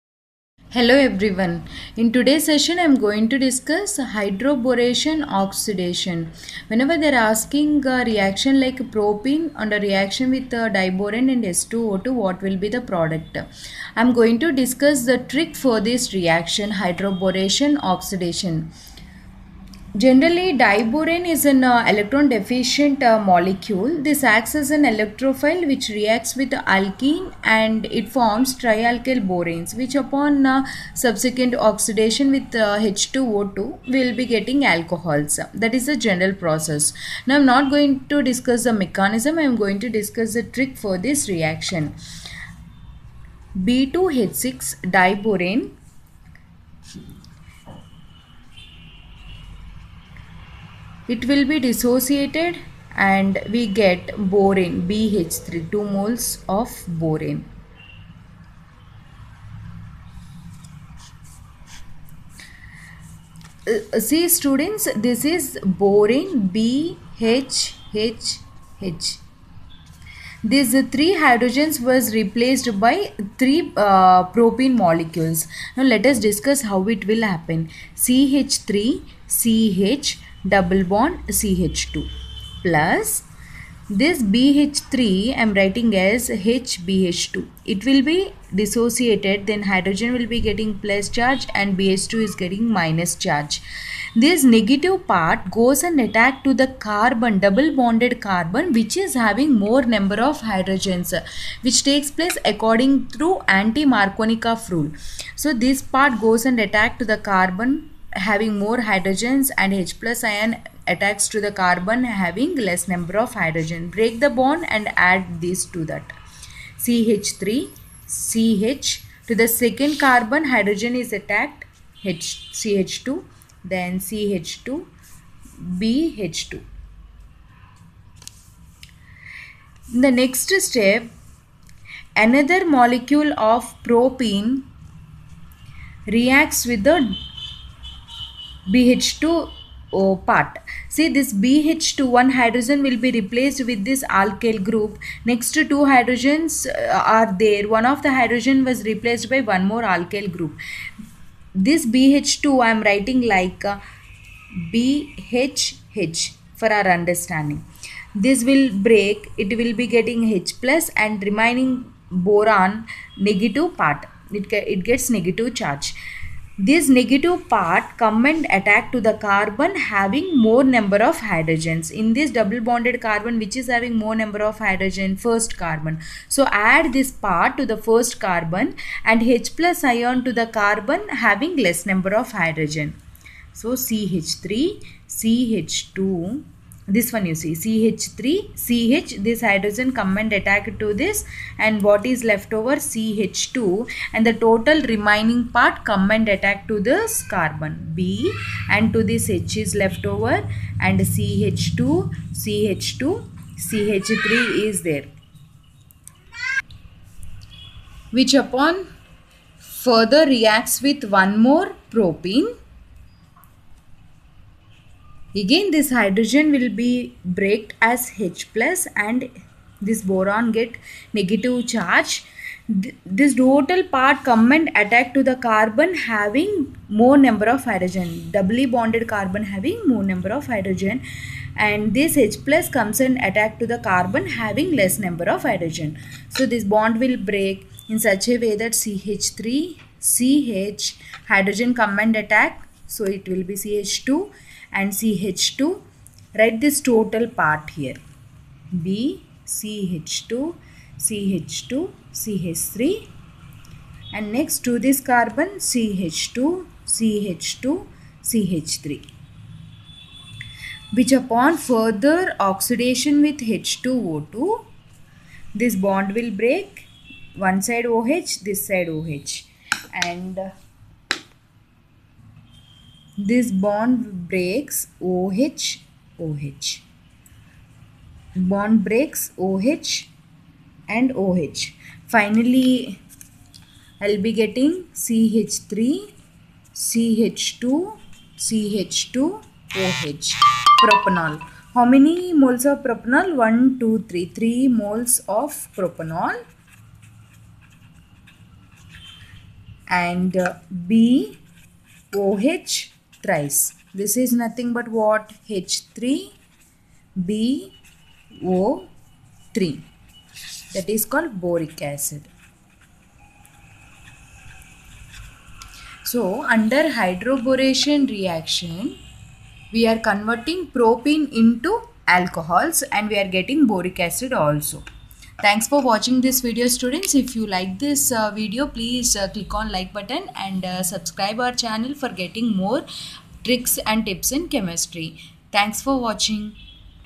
Trick For HydroBoration Oxidation
In this video I explained Trick For HydroBoration Oxidation which is an important concept for various competitive exams like IIT JEE, NEET, CET etc. To get more tricks subscribe my channel.